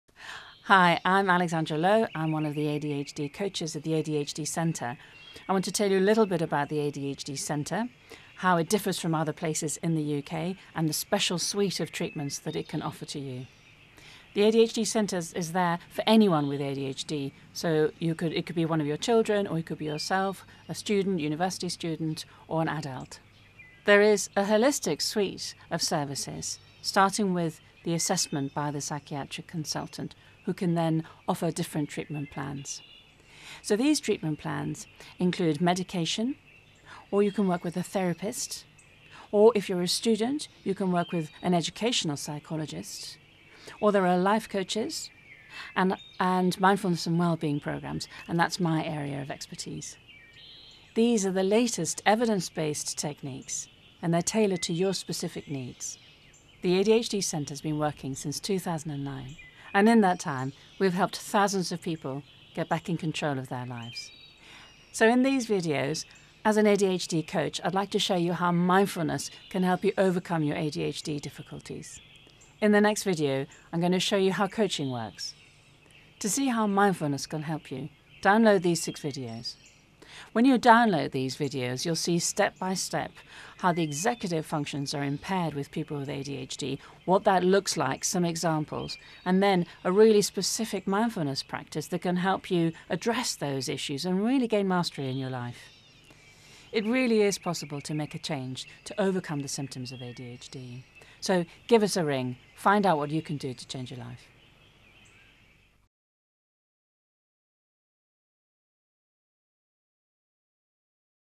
ADHD Treatment Plans - The ADHD Centre's Different Treatment Plans
Watch my "ADHD Treatment Plans" video and learn how the ADHD Centre differs from other places in the United Kingdom and is for anyone with ADHD. Discover the process of treatment plans and how the centre can help you overcome your ADHD difficulties.

You can also keep up to date with news and developments in a number of ways: